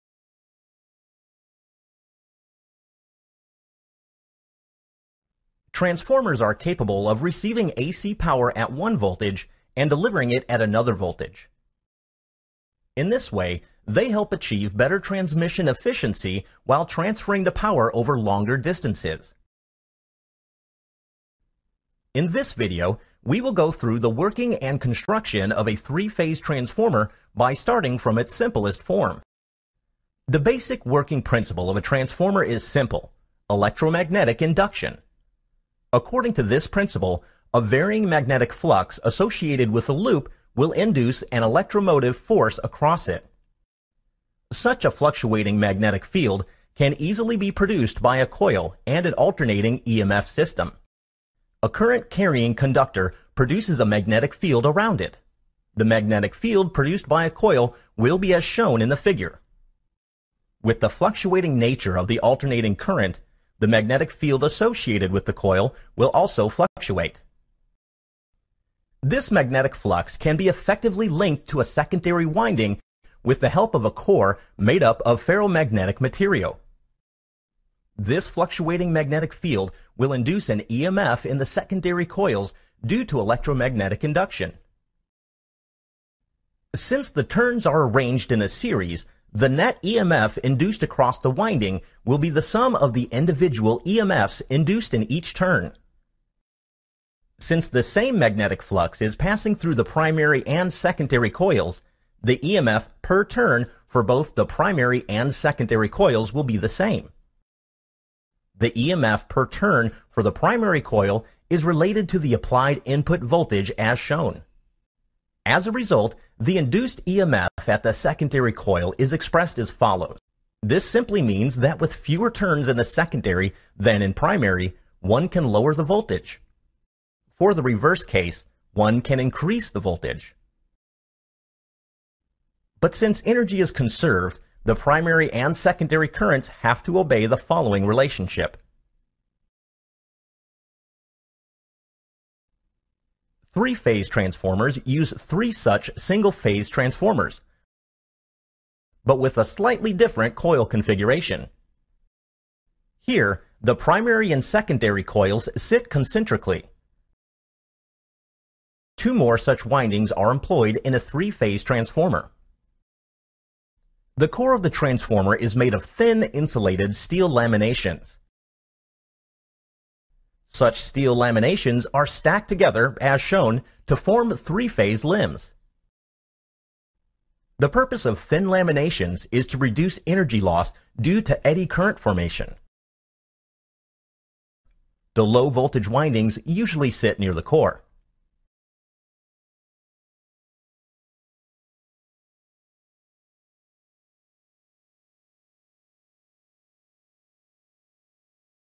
How Does A Transformer Work?
A basic explanation of the construction and working of a transformer.